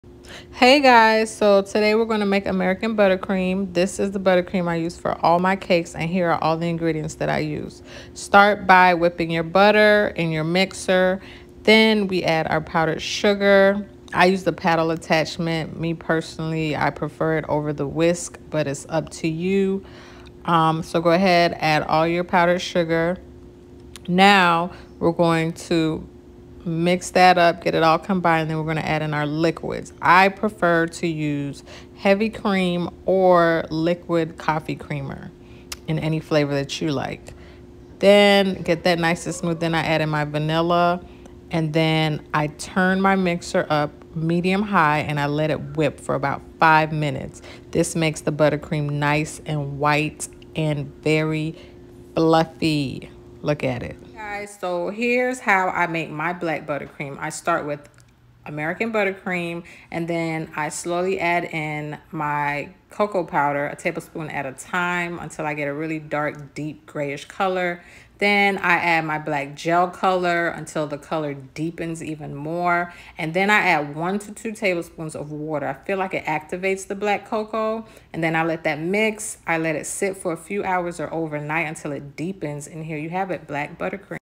How to Make American Buttercream & Black Buttercream | Cake Queen Tanya
1 Batch of My American Buttercream Recipe

1 lb (4 sticks) Butter
2 lbs (8 cups) Powdered Sugar
1 tsp of vanilla or preferred flavoring
1/3 cup heavy cream, liquid coffee creamer or liquid of choice

Add more or less liquid depending on the consistency that you would like

I usually double this recipe which makes 2 batches

Black Buttercream

Add a couple tablespoons of Black Cocoa Powder to American Buttercream
Then add a few drops of Americolor Super Black until a deep greyish black color forms
Add in a small amount of water
Mix all together in mixer
Cover and let sit until the color deepens
Tastes just like oreos!!

Black Cocoa Powder

Facebook: The Cake Queen Tanya